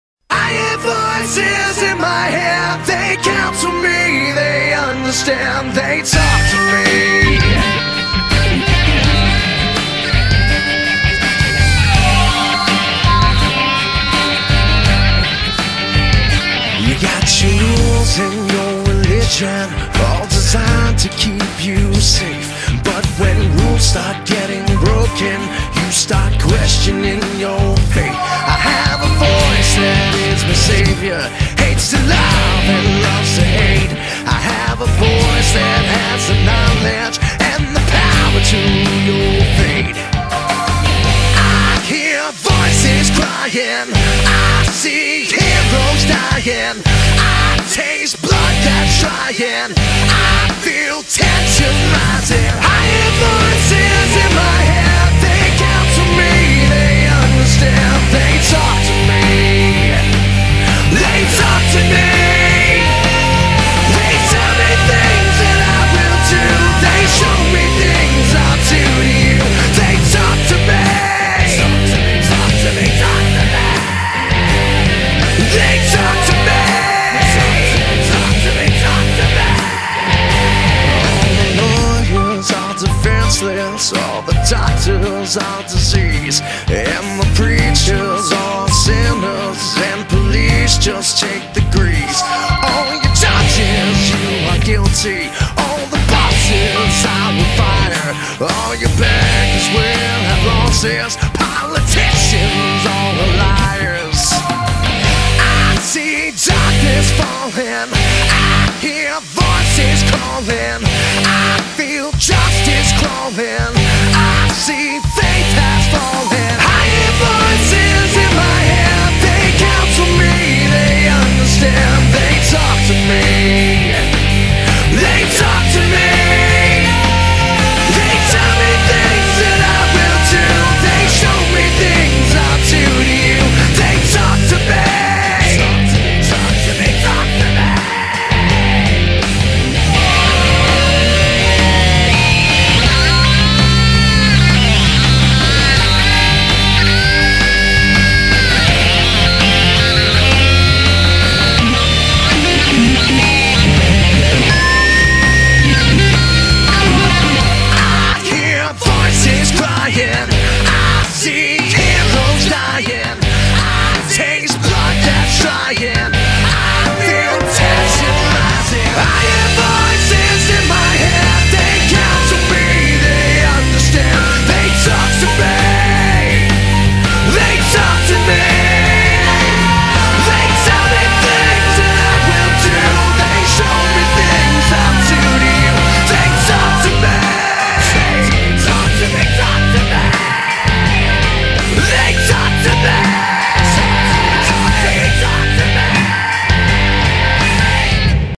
Randy Orton Custom Titantron
WWE Randal Keith Orton
I got lazy with this & Sony Vegas wasn't working. :P First video solely using CyberLink PowerDirector v10.